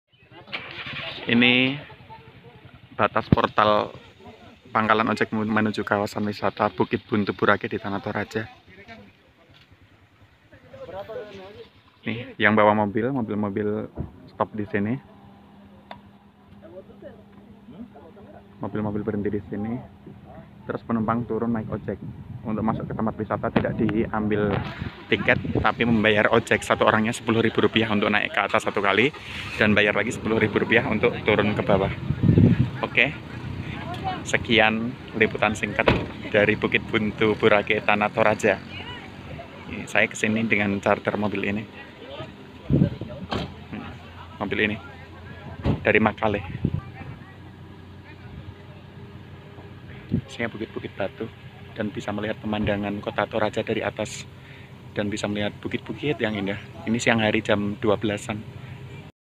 Ojek Wisata Buntu Burake di Tana Toraja | Toraya Maelo | Visit Indonesia Visit Sulsel
Tiket wisata: gratis
Naik bayar ojek Rp10k
Turun bayar ojek Rp10k